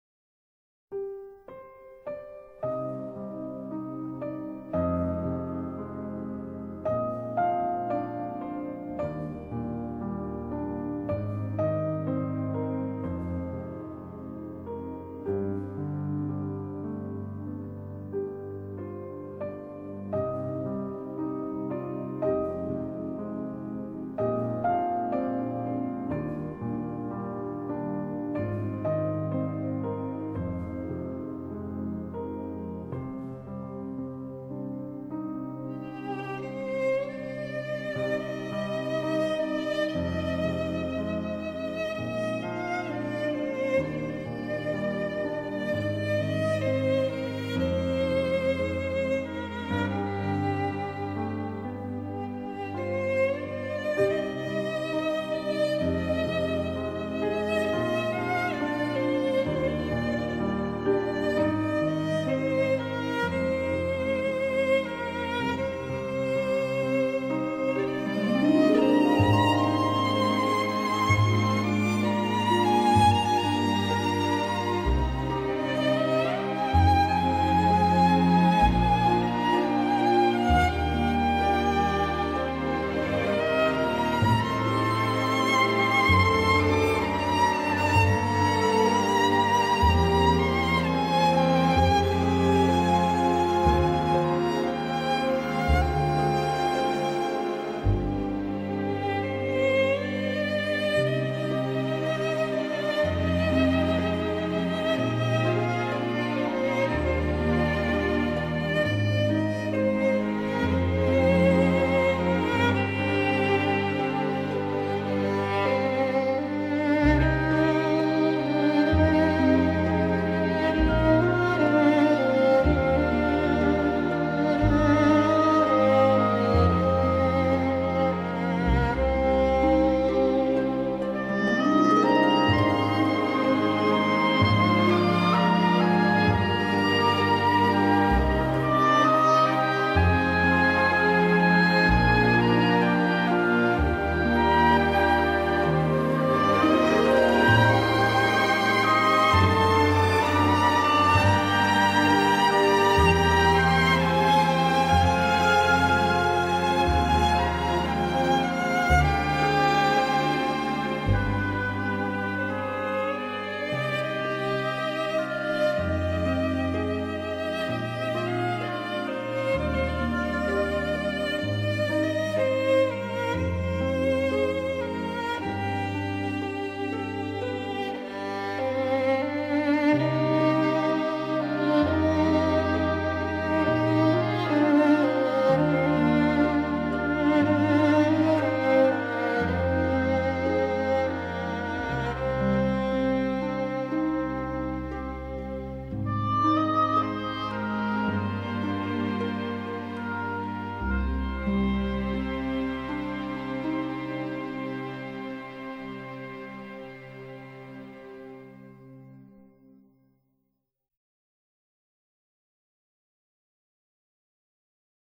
Kovács Sándor - Imádkozzunk című verse zenei aláfestéssel
Zene - Secret Garden